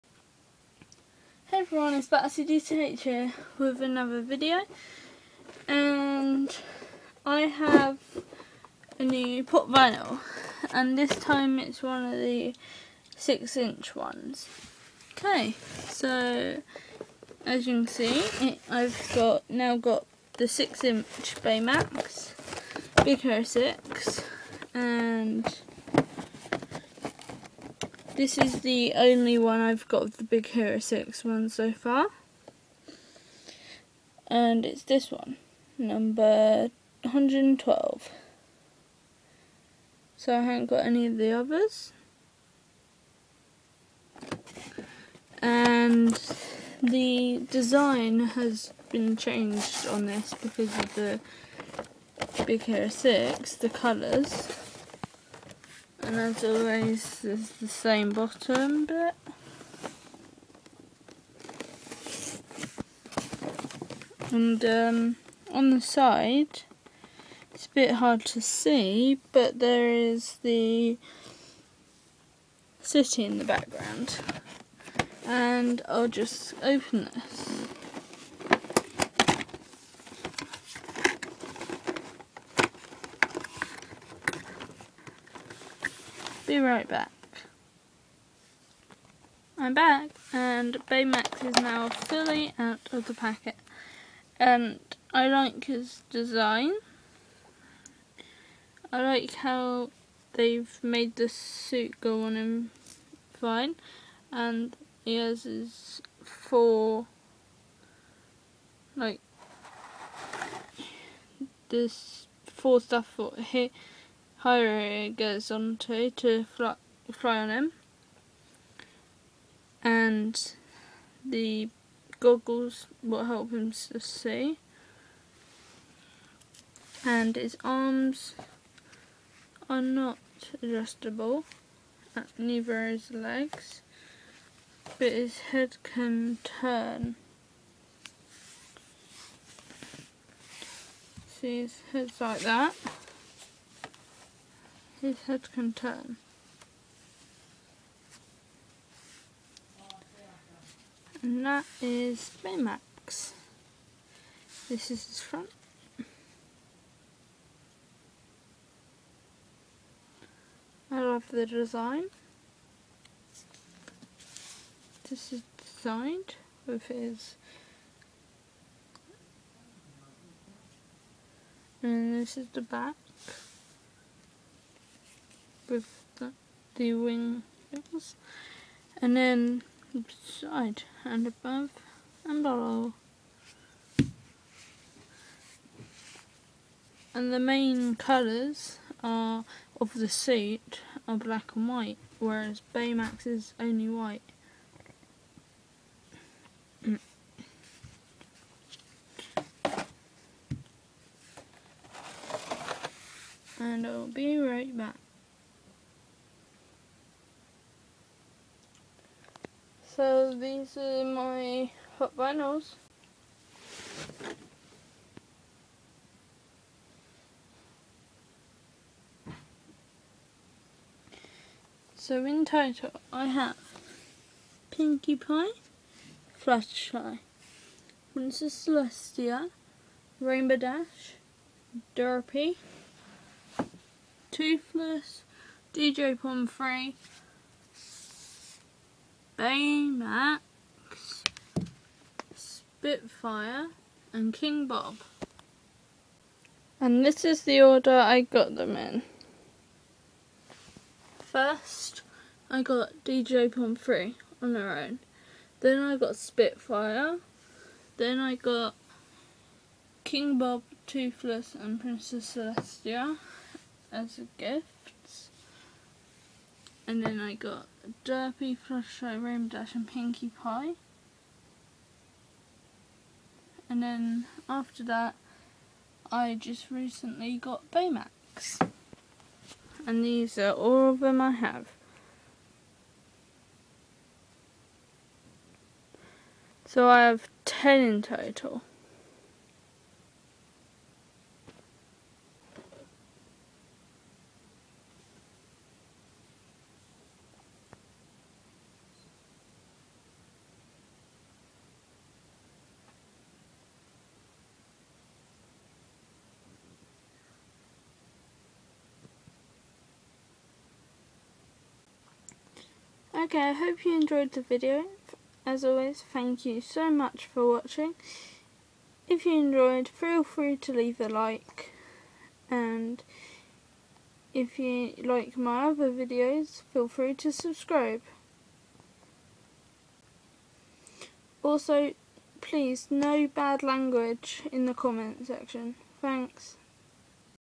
Big hero 6 Funko pop vinyl toy unboxing + review video
Today, I record a new video of me opening my new baymax pop vinyl!!! I love the pop vinyls and I try to make a video every time I get a new one!

Please no swearing or inappropriate language in the comment section, thanks you!

Also, hope everyone has an amazing day!!!